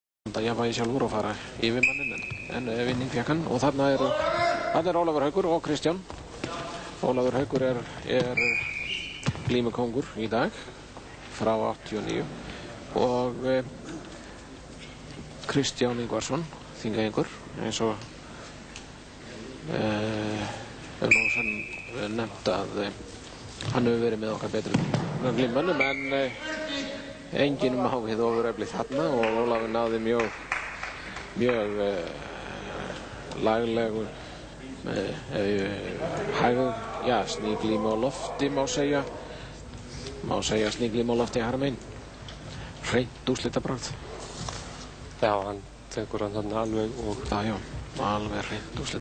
Icelandic Glima Championship 1990 Kristján Yngvasson vs Ólahfur H Ólafsson
Icelandic Glima Championship Trournament 1990 for the Grettisbelt Recodring from the Icelandic national Television